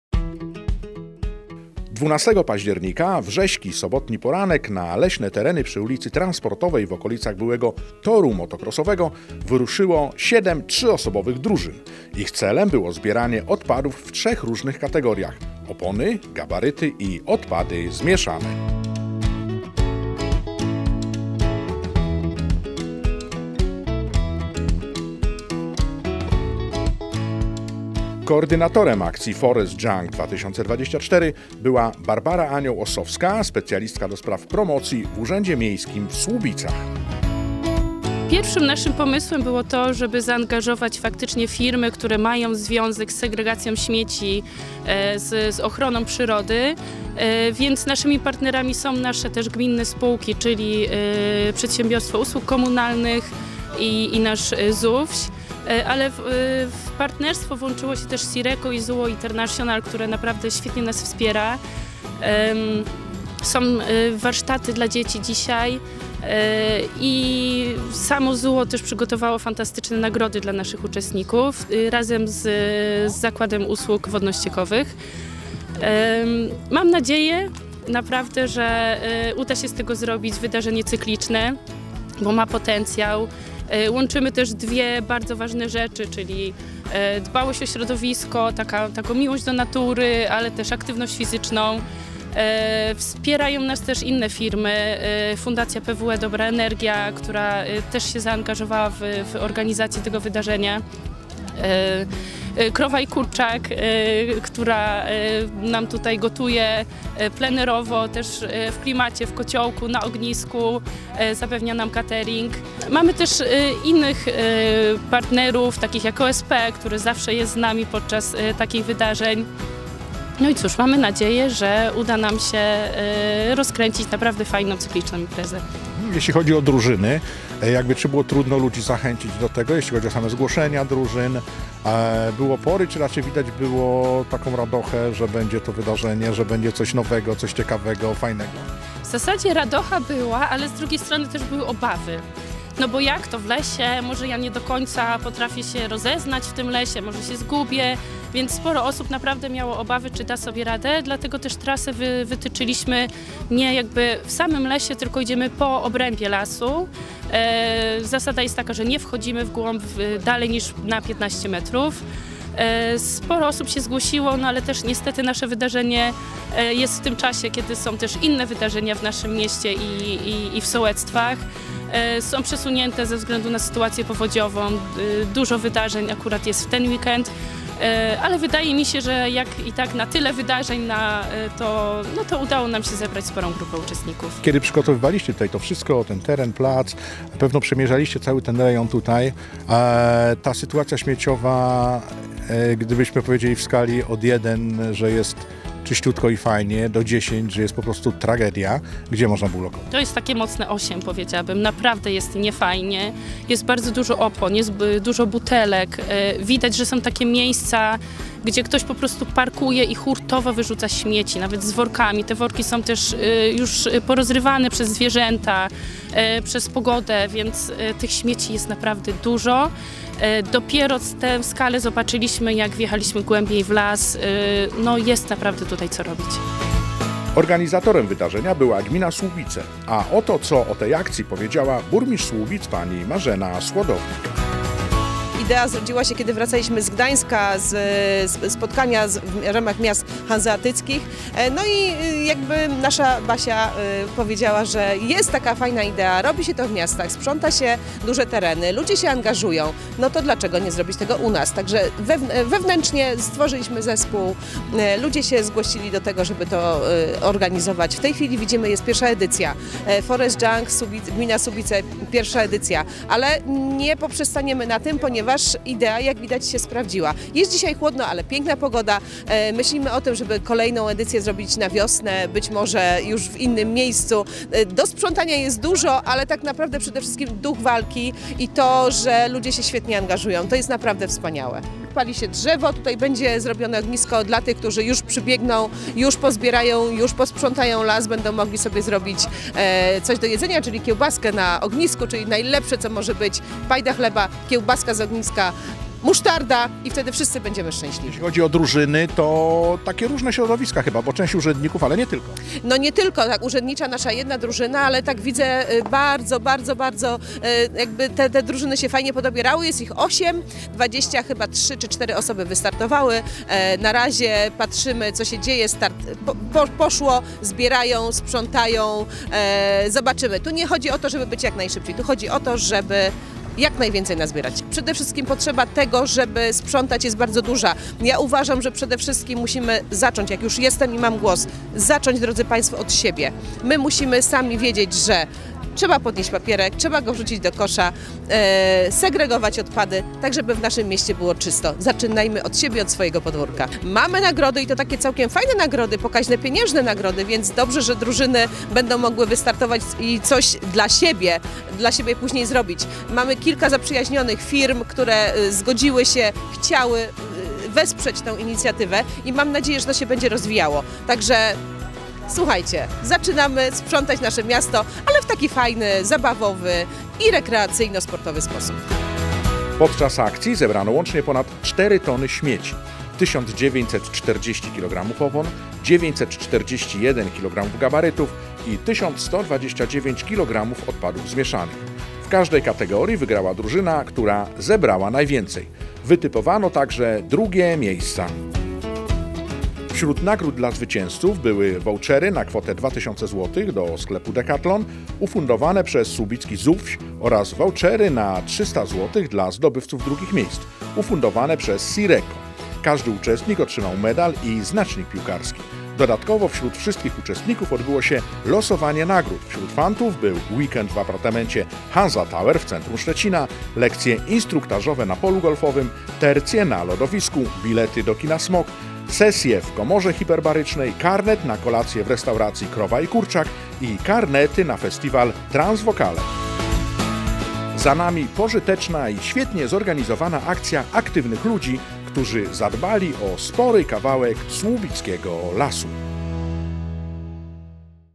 Forest Junk 2024 - pożyteczna akcja aktywnych ludzi
12 października w rześki sobotni poranek na leśne tereny przy ul. Transportowej wyruszyło siedem trzyosobowych drużyn. Byli to uczestnicy akcji zorganizowanej przez Urząd Miejski w Słubicach - Forest Junk 2024